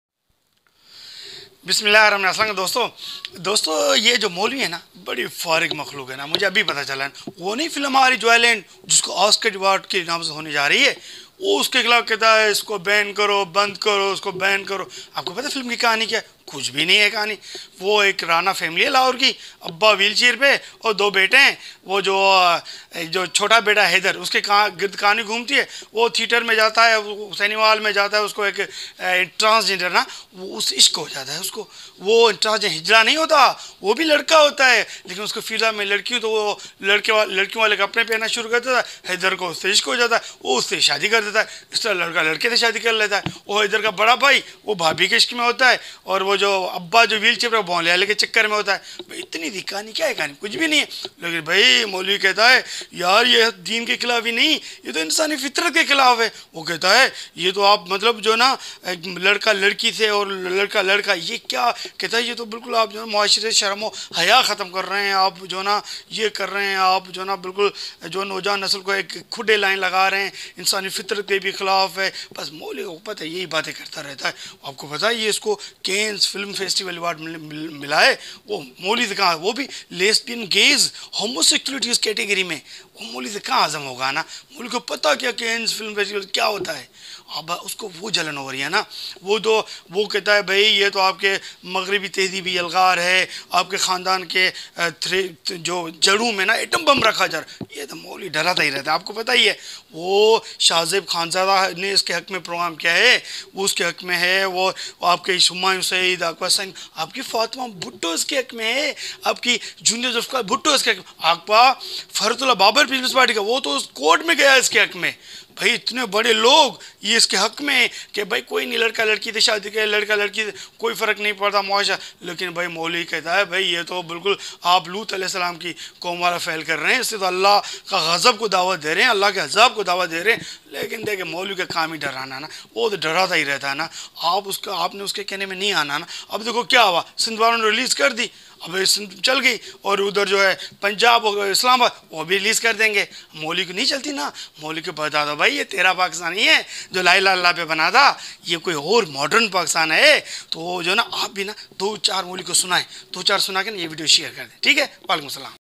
Joy Land Movie Vs Pakistan's Molvi || Family System of islam & joy Land || Muslim Soceity & Film
Many voices raised in Pakistan to ban the Joyland movies.This video is mocking on the Joy land film. Apparently you this is in the favour of film but just mockery.